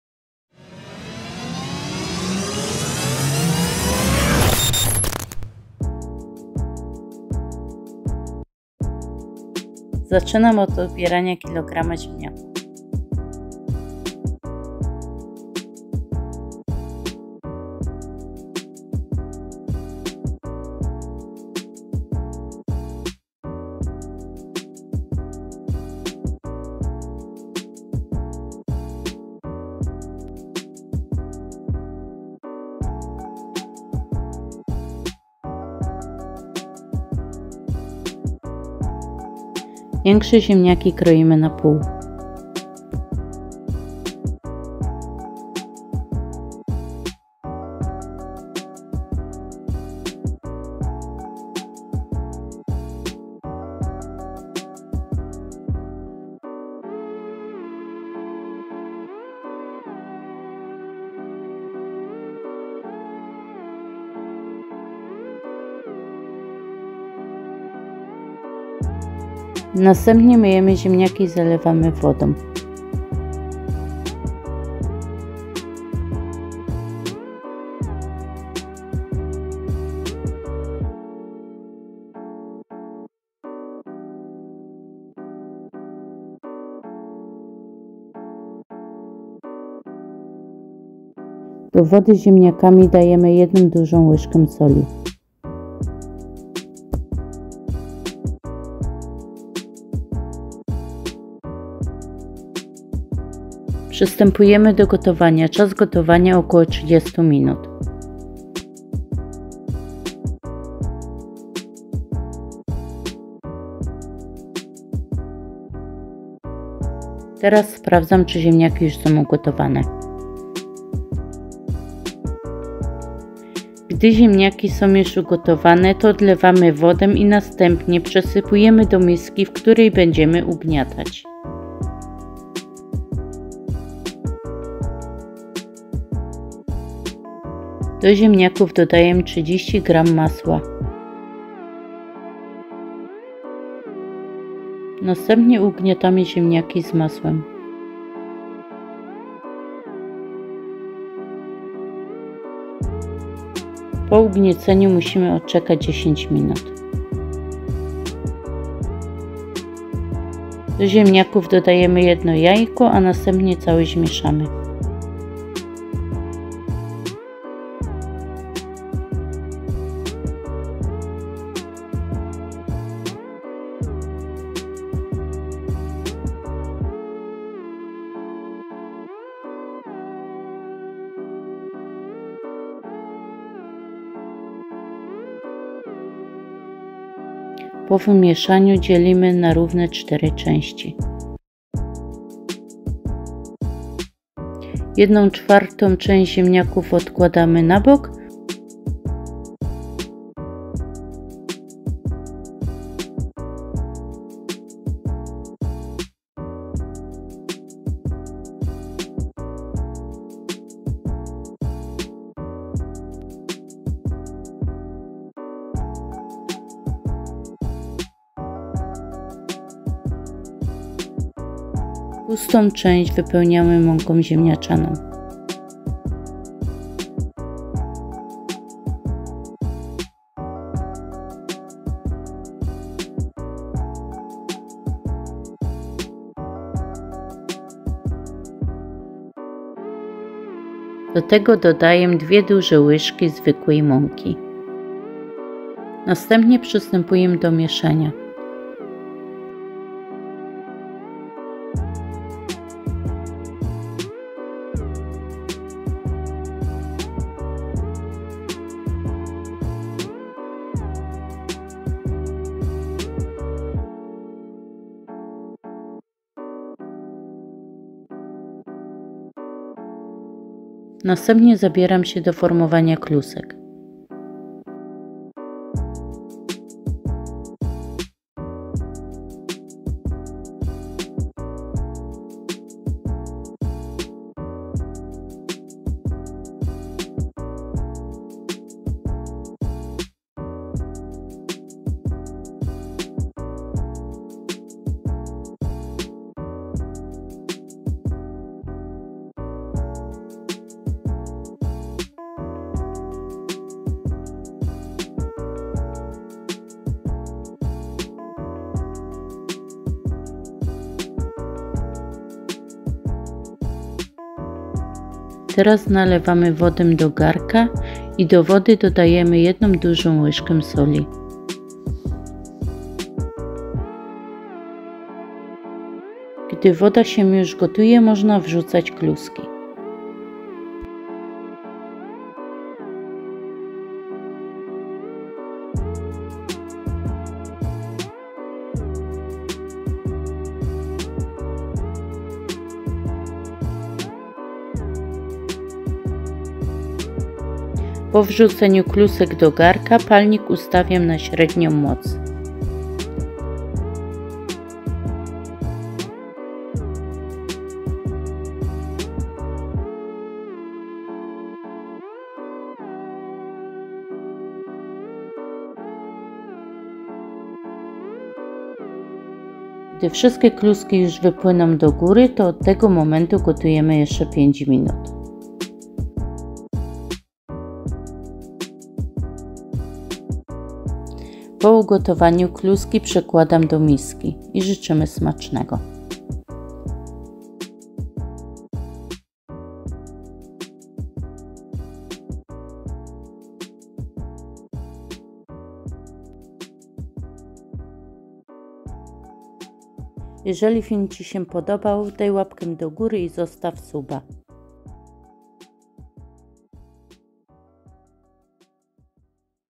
Przepis na Śląskie Kluski
Tak moja żonka robi najlepsze na świecie Kluski Śląskie.